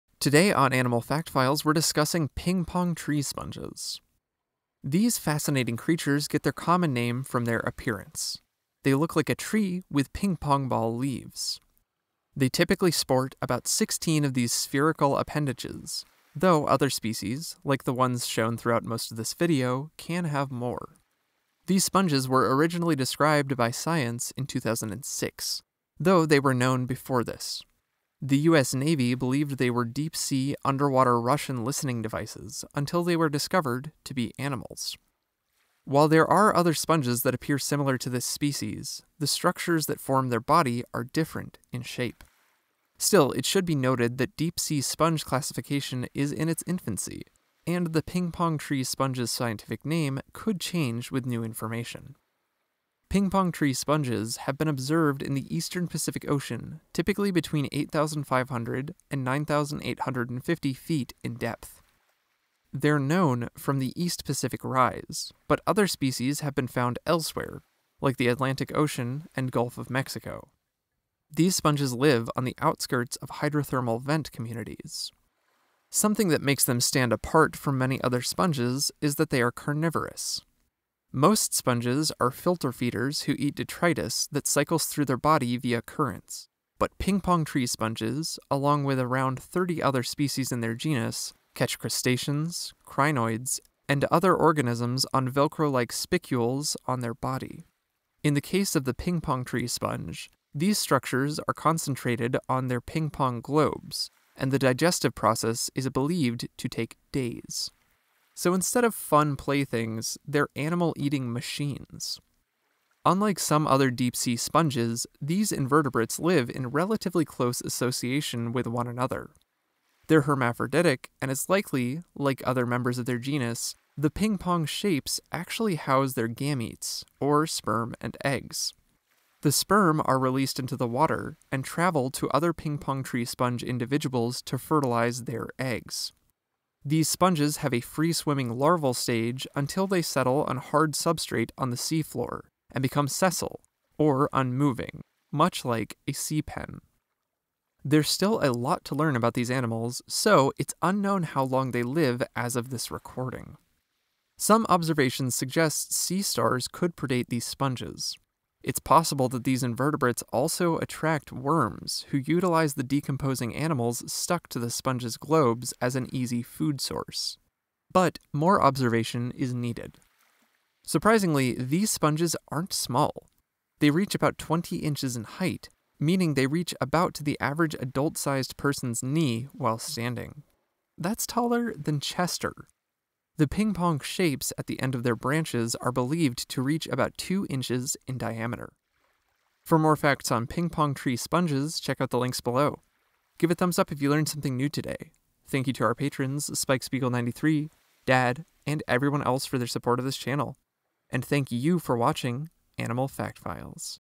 Ping Pong Tree Sponge Facts: a PLAYFUL CARNIVORE 🧽 Animal Fact Files
Though the ping pong tree sponge is, in fact, a sponge, it is also a playful carnivore! These carnivorous sponges eat other animals instead of filter feeding and they use their ping pong ball structures to capture prey. Learn more about the ping pong tree sponge on this episode of Animal Fact Files!

Scientific Name: Chondrocladia lampadiglobus, and others of the genus
Range: Eastern Pacific Ocean, possibly elsewhere
Habitat: outskirts of hydrothermal vent communities
Size: 20 inches (50cm) tall
Diet: crustaceans, crinoids, other animals
Threats: possibly sea stars
Lifespan: unknown

Ping Pong Tree Sponge APPERANCE 0:00
Ping Pong Tree Sponge DISCOVERY 0:20
Ping Pong Tree Sponge HABITAT 0:52
Ping Pong Tree Sponge DIET 1:15
Ping Pong Tree Sponge LIFE CYCLE 1:52
Ping Pong Tree Sponge THREATS 2:27
Ping Pong Tree Sponge SIZE 2:50
Thanks! 3:10

Image and Video Credits:
Videos from NOAA
Van Soest RWM, Boury-Esnault N, Vacelet J, Dohrmann M, Erpenbeck D, De Voogd NJ, et al. (2012) Global Diversity of Sponges (Porifera). PLoS ONE 7(4): e35105.